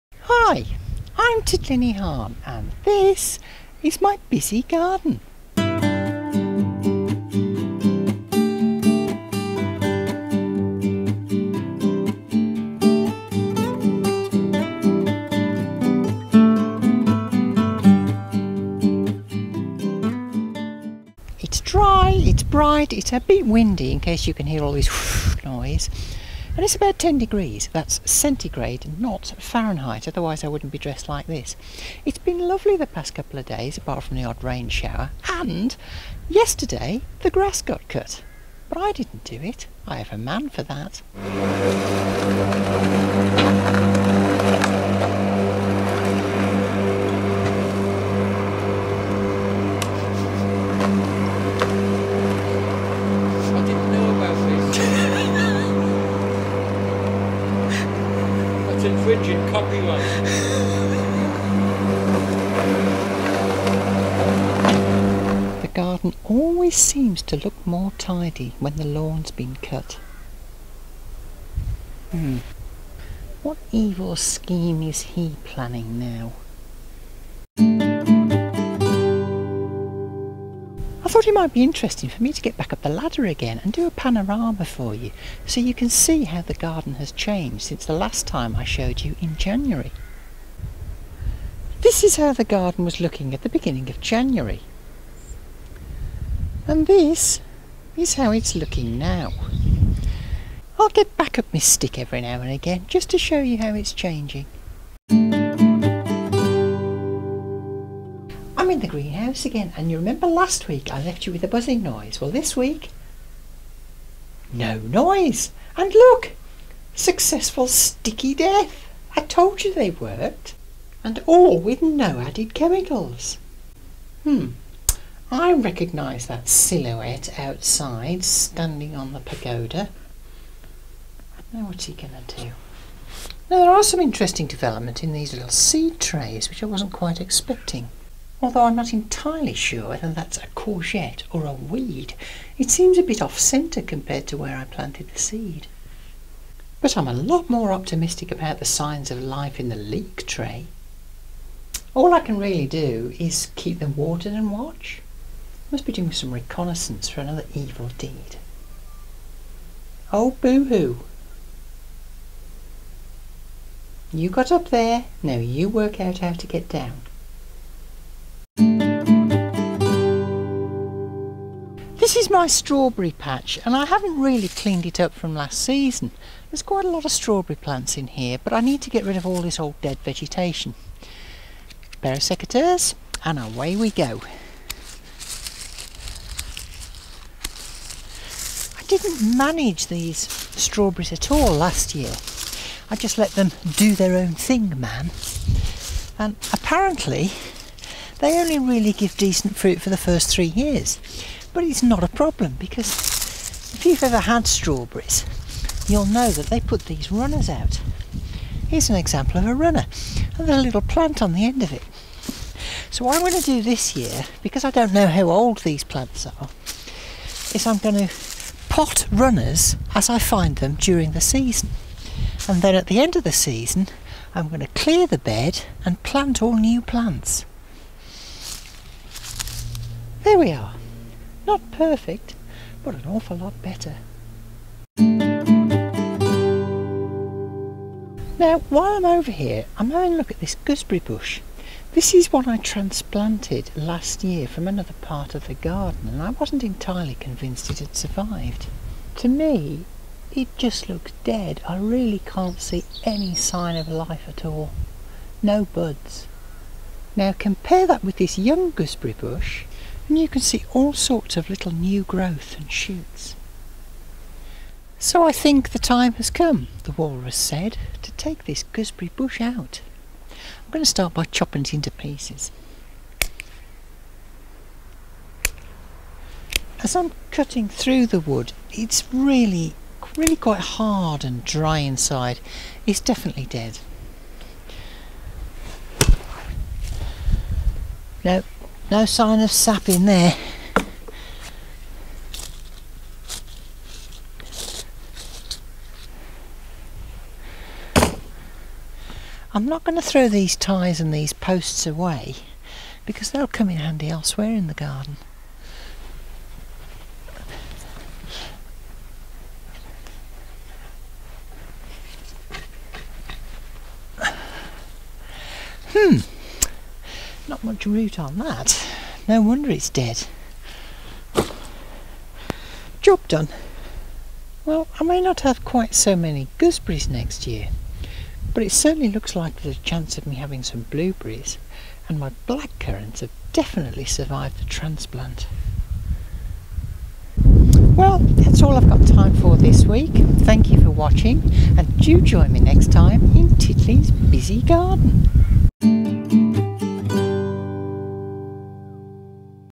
Titli's Busy Garden 2012 Week 10 - Back in the Fruit Patch
This week I sort out my strawberry patch and take action with my transplanted gooseberry bush.